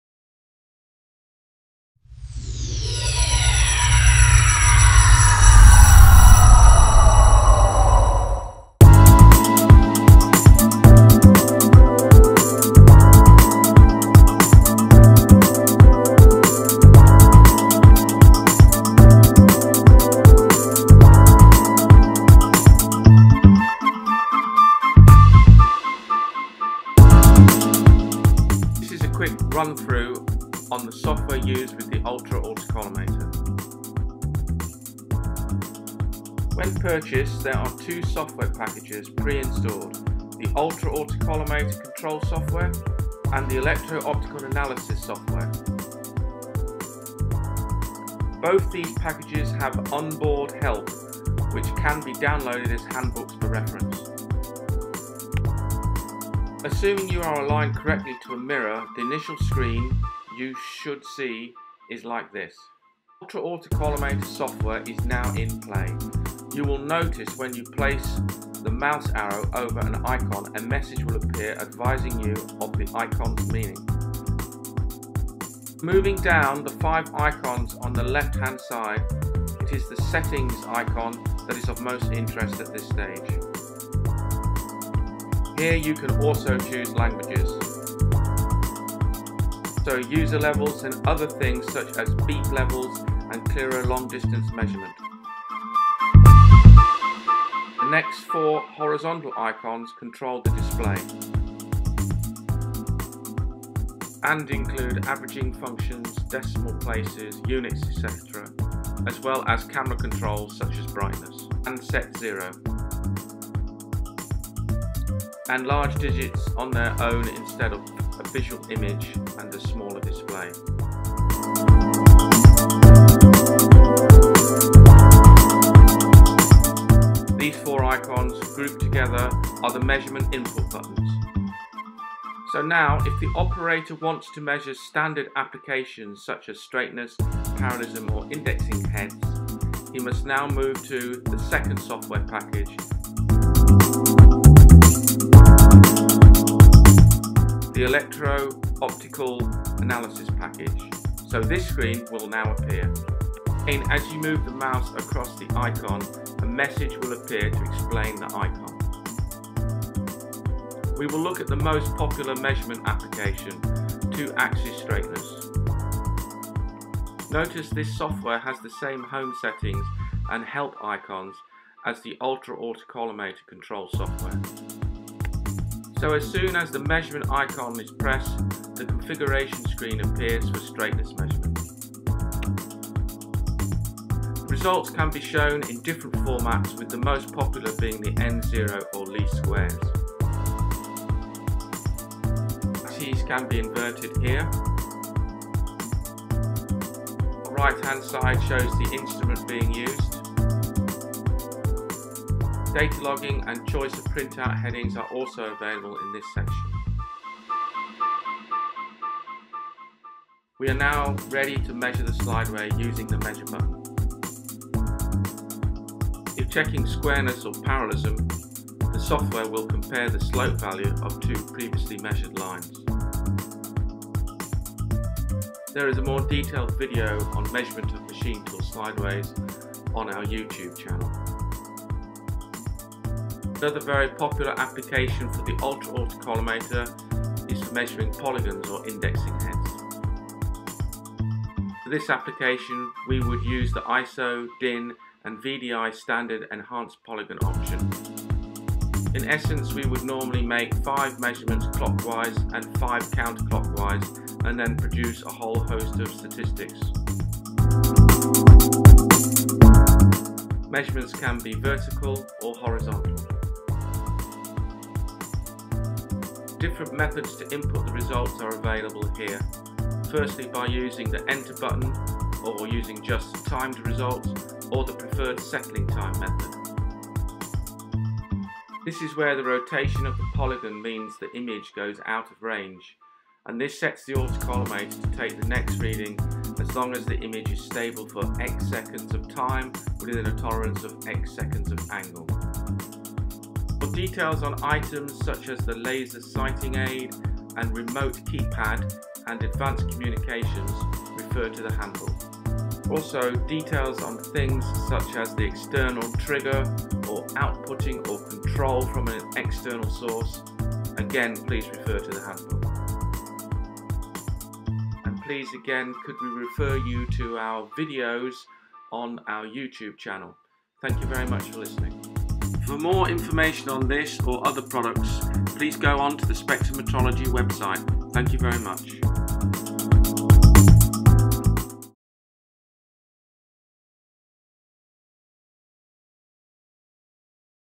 Ultra autocollimator software tutorial
This tutorial shows the latest Taylor Hobson ultra autocollimator software and explains the functions of the various software options.